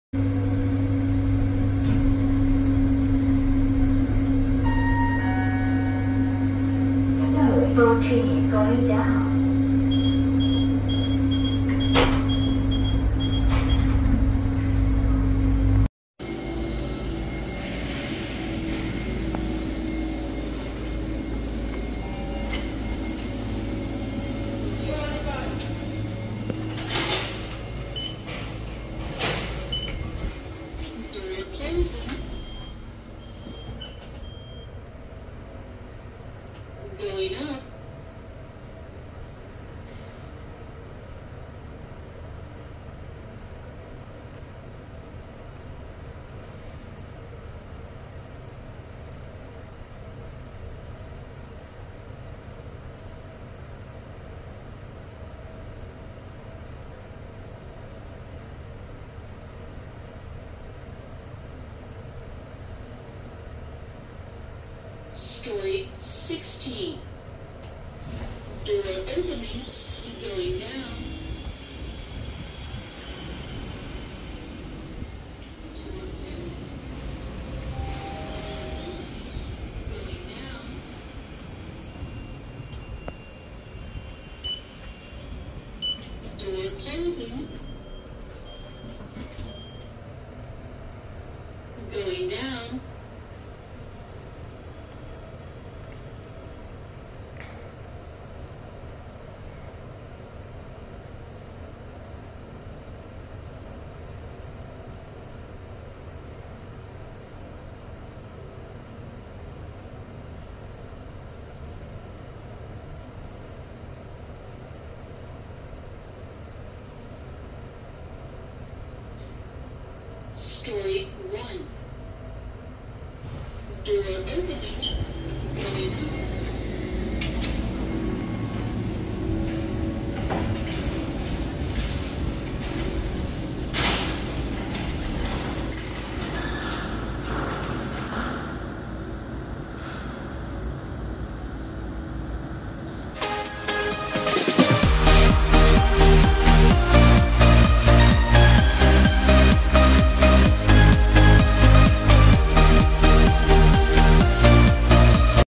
Mitsubishi lifts at Blk 119B Alkaff Breeze
And of course, the one which I saved the best for last in the Alkaff series is the Alkaff Breeze BTO using 31st term Mitsubishi lifts with the same Navy blue doors and oblong shaped indicators and buttons as Champions Green but only 6 floors taller. Oddly enough, despite being only ONE floor less than the other Alkaff BTOs with iFE lifts and having the exact same floor count as the 28th term Hitachi at Woodleigh Hillside, the lifts here have a machine room as you can tell by the counterweight placed at the back and the lack of an E&I panel.

Manufacturer: Mitsubishi
Floors: 1, 2, 3, 4, 5, 6, 7, 8, 9, 10, 11, 12, 13, 14, 15, 16
Capacity: 13 persons 885kg
Model: NEXIEZ-MR
Year installed: 2024
Rating: 50/50
History: Original

Camera: Samsung Galaxy S23 Ultra
Date filmed: 30 July 2024